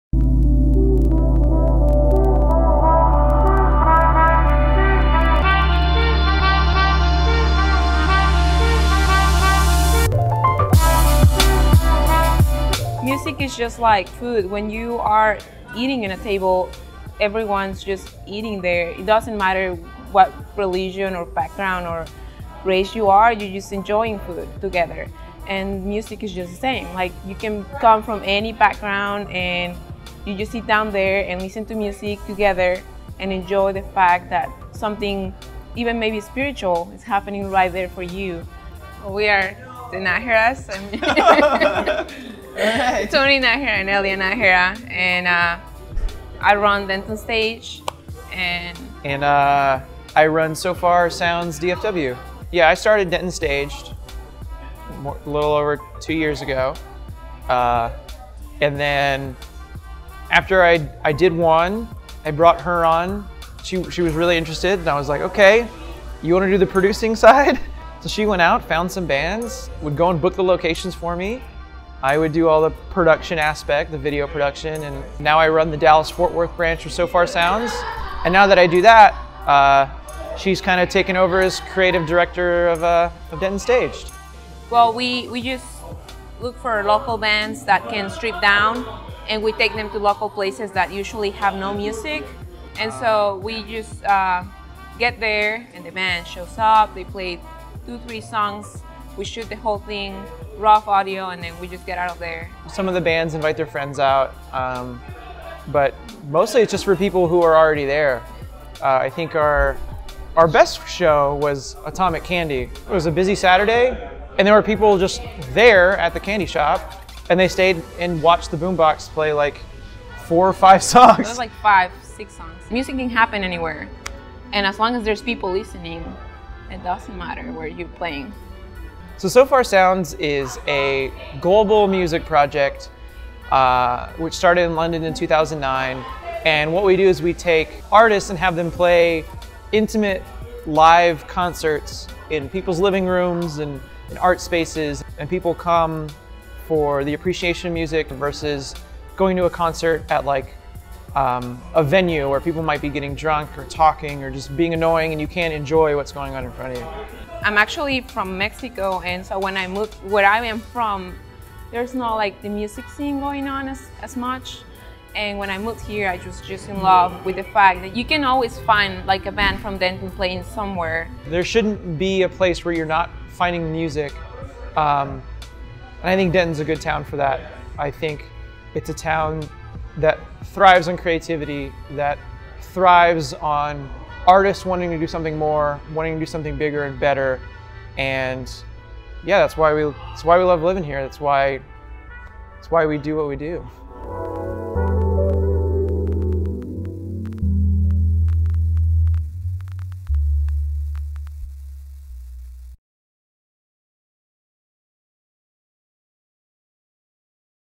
Denton Does: MUSIC with Tony & Elia Najera
In a time when venue after venue closes in Denton, Tony and Elia Najera work to take music new and interesting places. If you’ve ever been treated to a surprise concert at your favorite Denton landmark, or a secret show in the DFW metroplex, you likely have one (or probably both) of them to thank.

SPECIAL THANKS TO:
The Bearded Monk